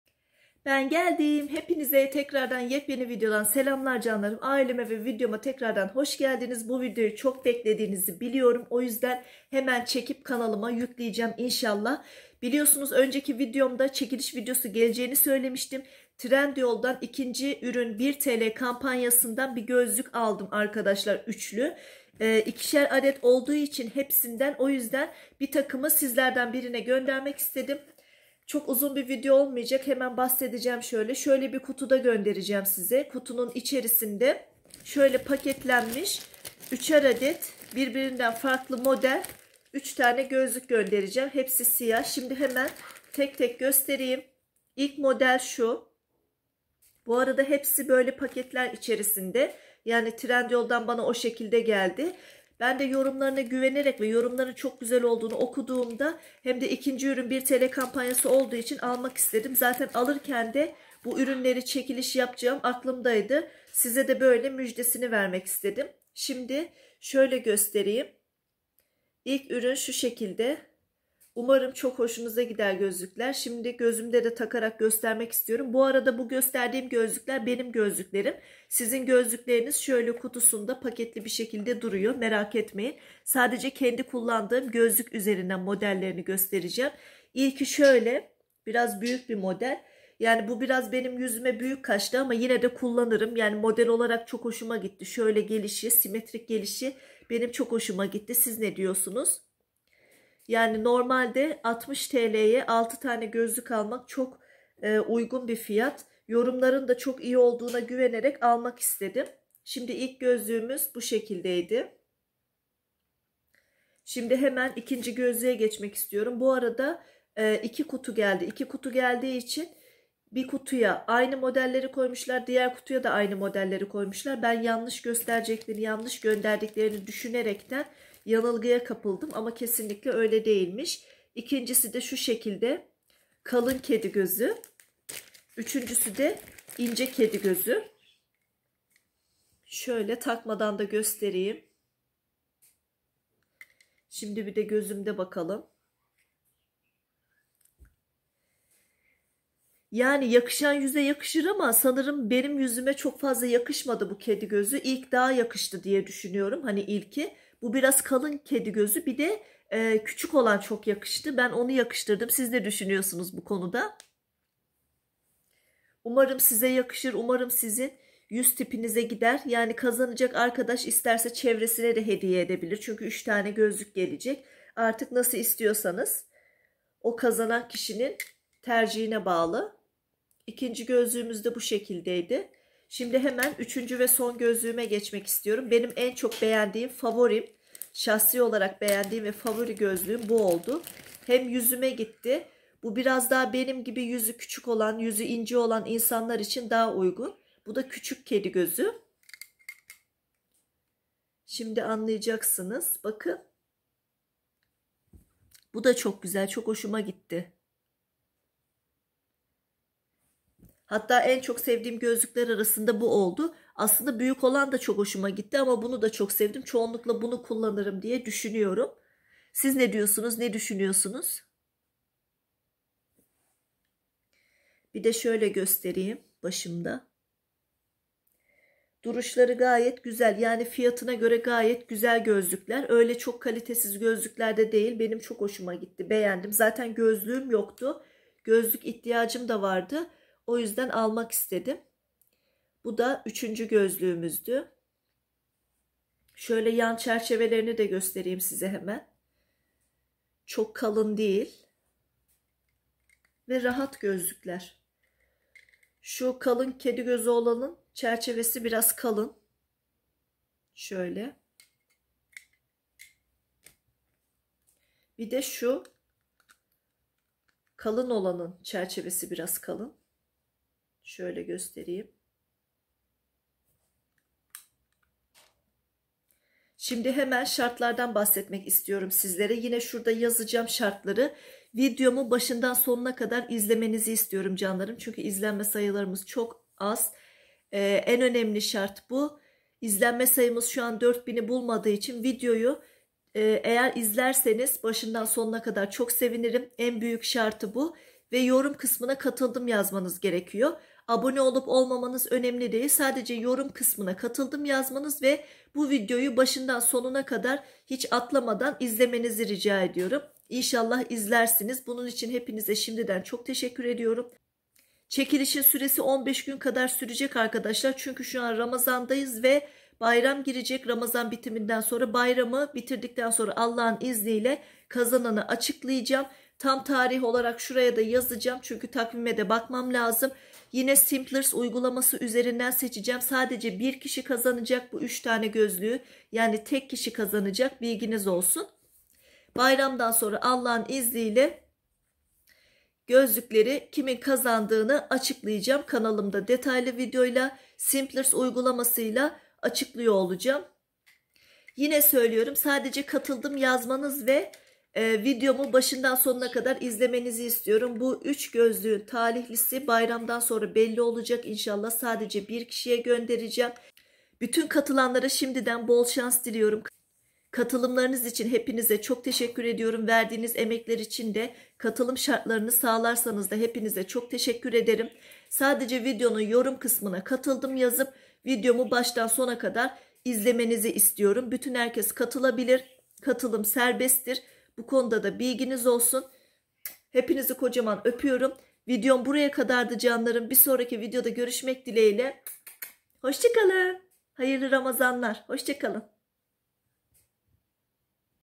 Trendyol Gözlük Çekilişi | Yeni çekiliş
Herkese selam canlarım
Yepyeni çekiliş videosuyla geldim..
Trendyoldan kampanyalı satin aldığım gozluklerden 1 takımı sizlerden birine hediye edeceğim..
3 ader farklı model sadece 1 kişi gidicek.
Herkese bol şanslar dilerim .

Katılım Şartları
Videomu sonuna Kadar izlemek
Yorum kısmına katıldım yazmak.

Sadece bu kadar ..
Sizleri seviyorum ..